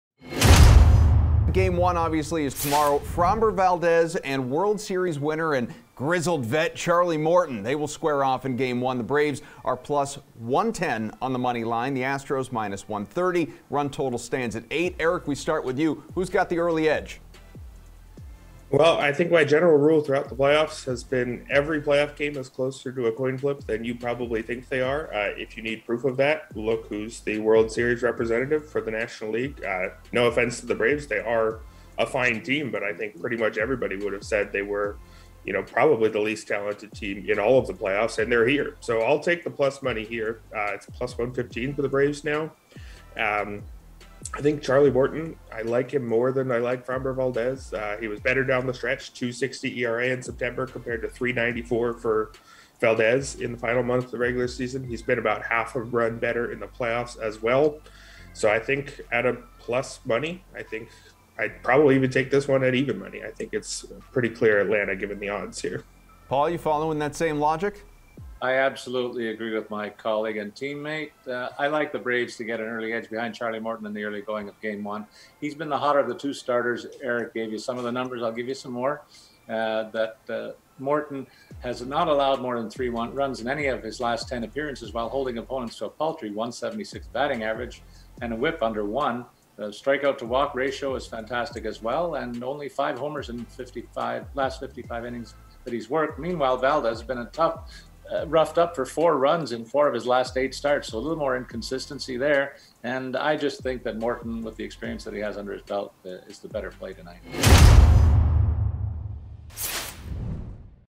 Does Atlanta or Houston have the edge in World Series Game 1?
Rotowire’s Erik Halterman and Paul Bruno join the Sweat to discuss who has the advantage ahead of Tuesday’s World Series Game 1 matchup between the Houston Astros and Atlanta Braves